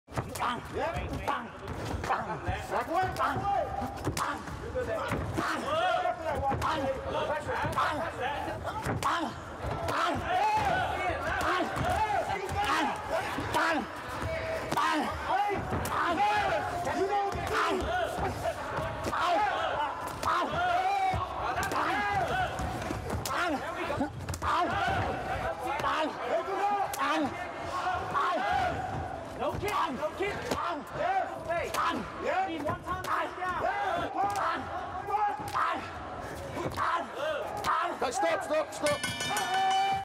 LISTEN TO THE SOUND OF THOSE KICKS 😰
Thai star Yodkaikaew Fairtex CRUSHED American warrior Alex Schild with an insane kicking display at ONE: A NEW BREED in 2020 😱